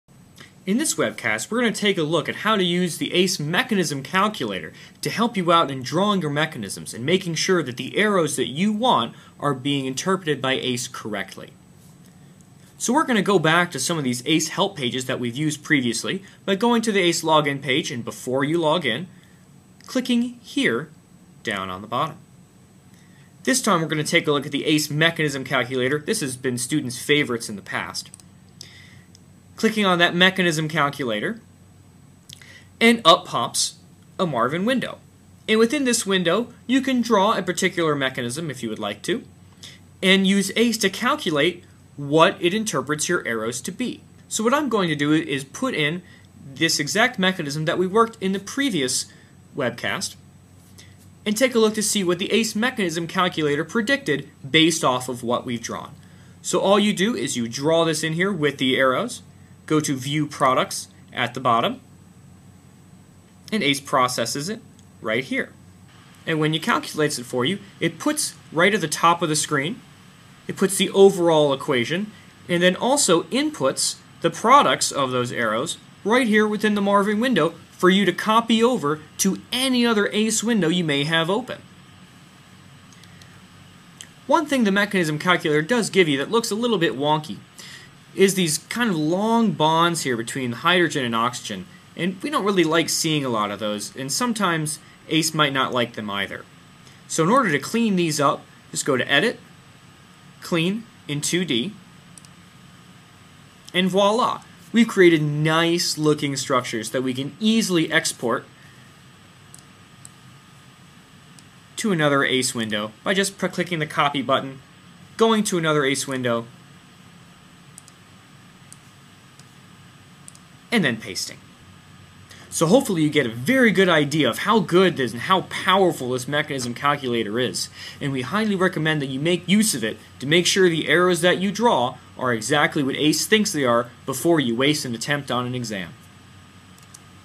The Mechanism Calculator Tool
This webcast and the accompanying website explains how to use the mechanism calculator in ACE Organic to check if your curved arrows are being properly interpreted.